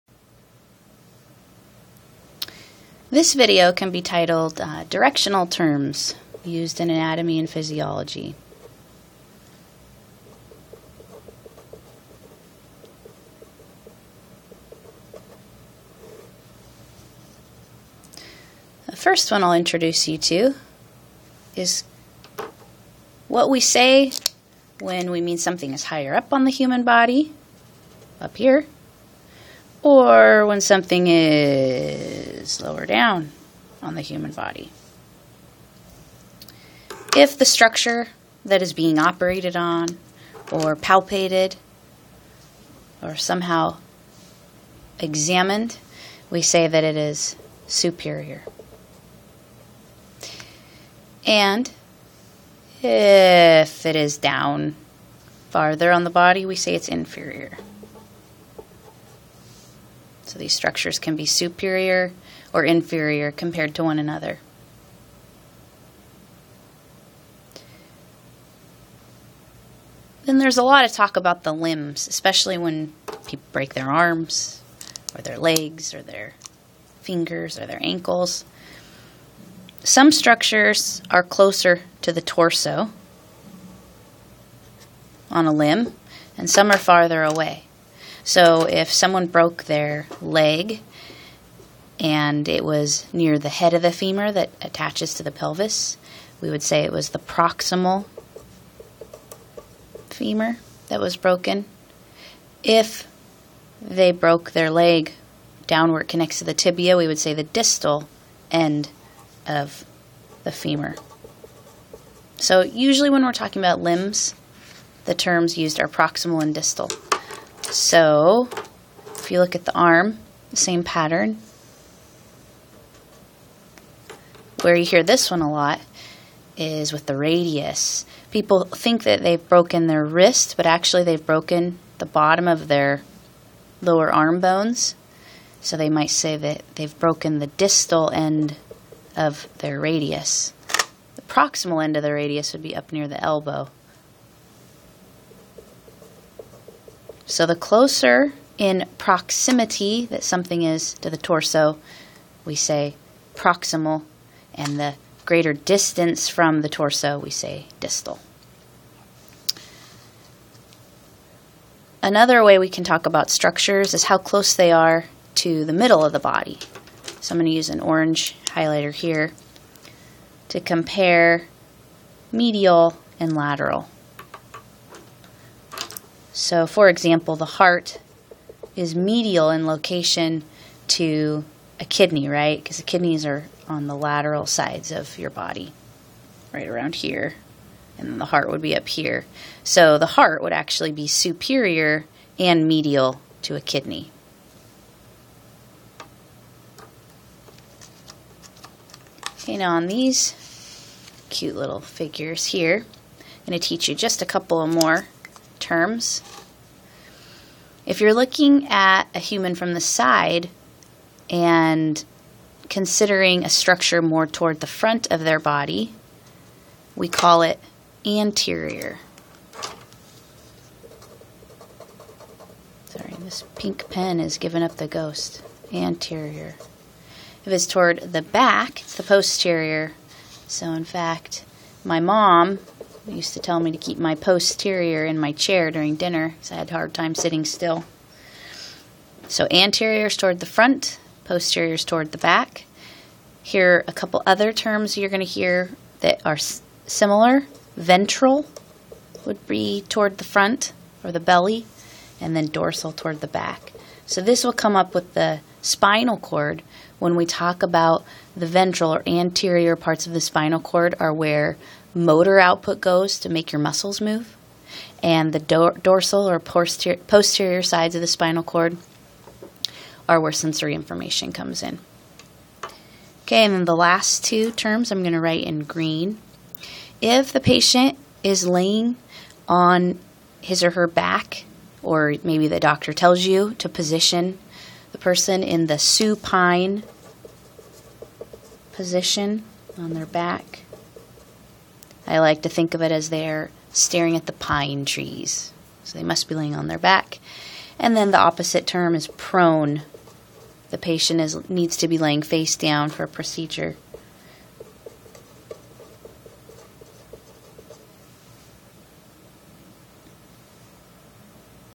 Directional Terms used in A&P
This is the first video I ever made for this channel! has diagrams, notes, and practice questions.